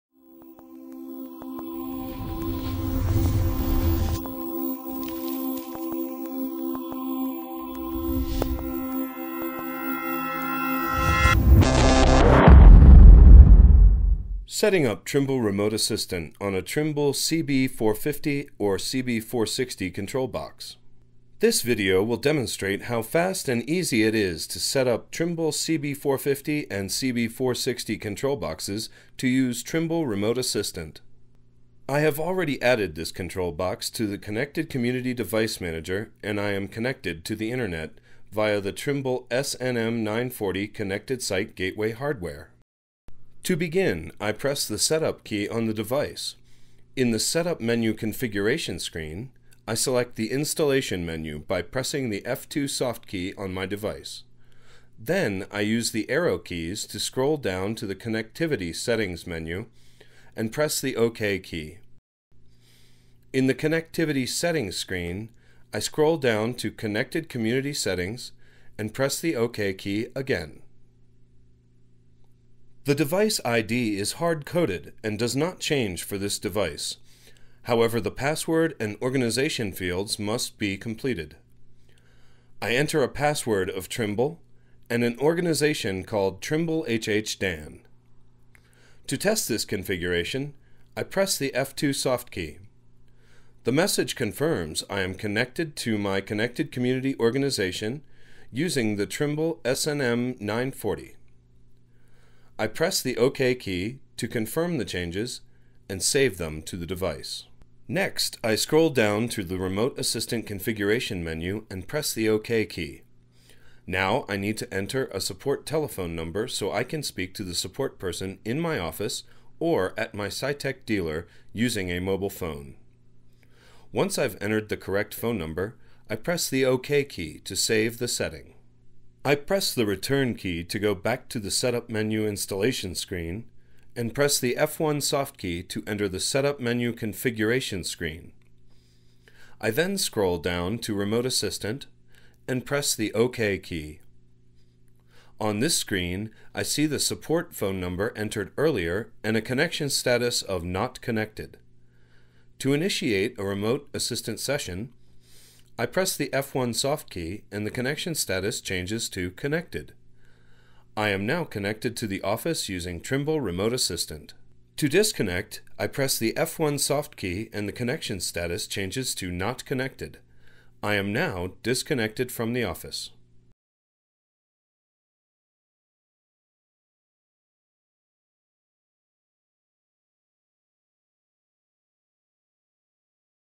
Setting up Trimble Remote Assistant on a CB450 or CB460.mp4
How to set up Trimble Remote Assistant using Trimble GCS900 Grade Control System version 12.4 and a Trimble CB450 or CB460 Control Box.